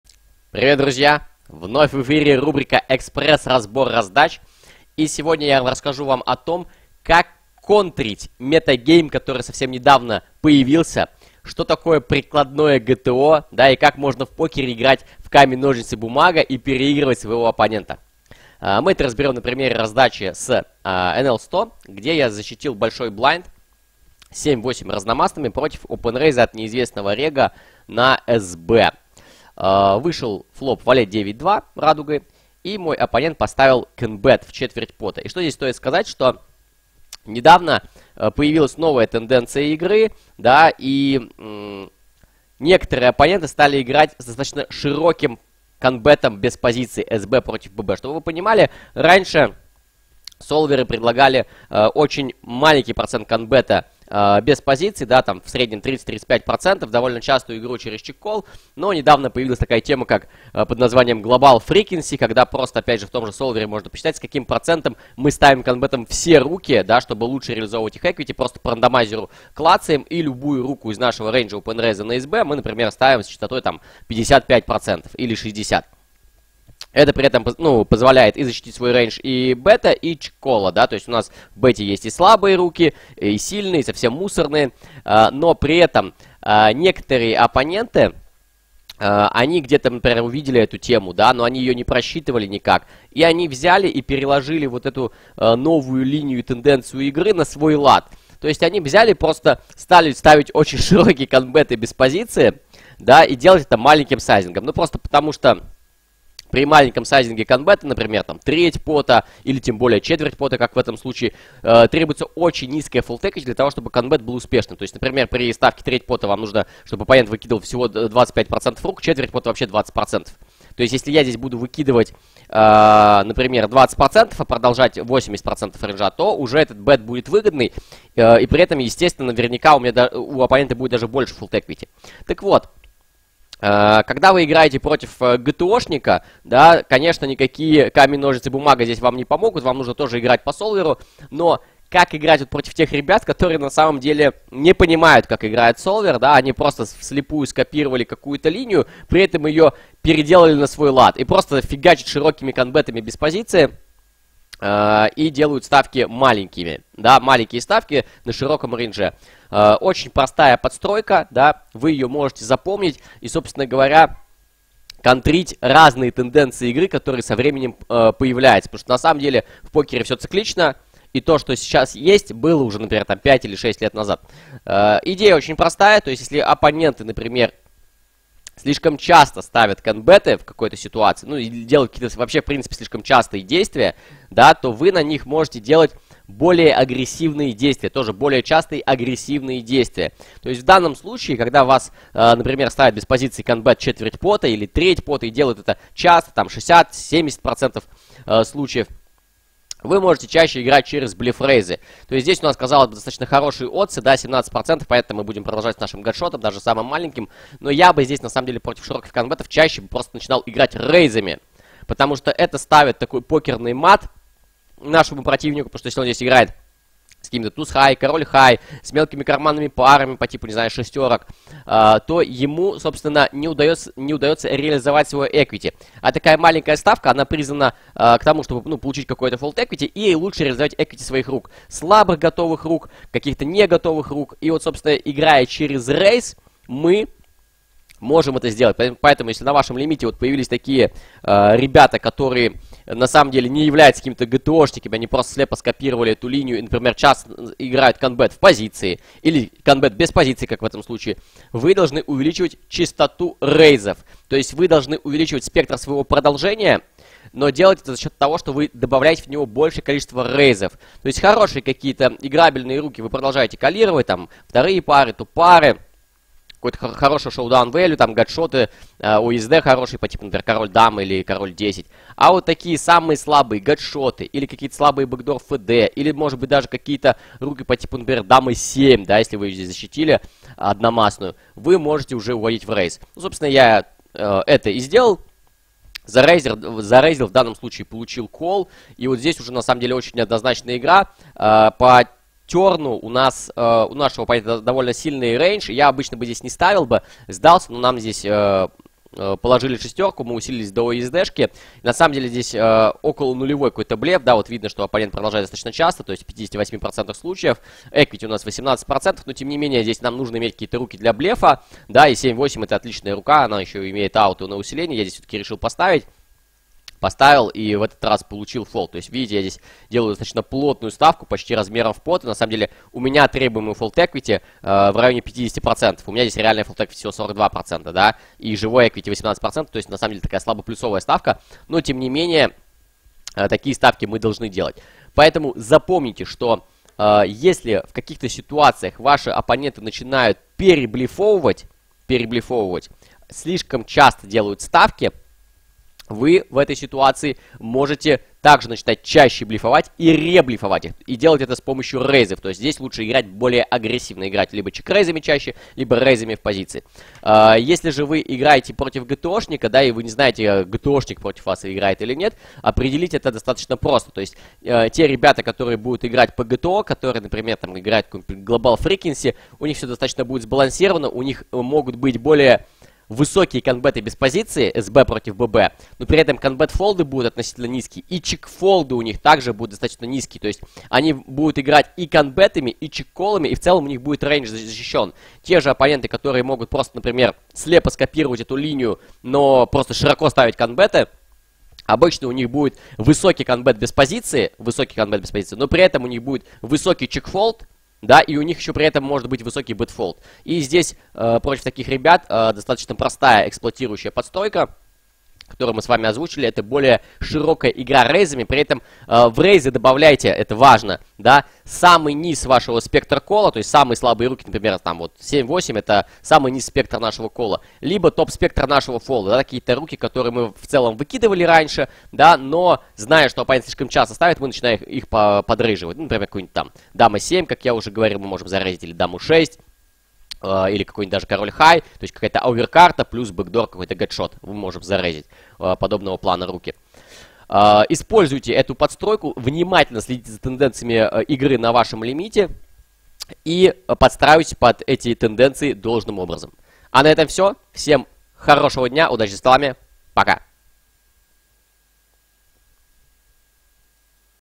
Как противостоять мамкиным "гто-шникам" с их кбетами по 1/3 или 1/4 пота когда у вас нет руки?
Разбор раздачи с игрой на BB против рега на SB который широко кбетит сайзом 1/4 пота.

18+: глобал фрикинси, мета-гейм и другие извращения.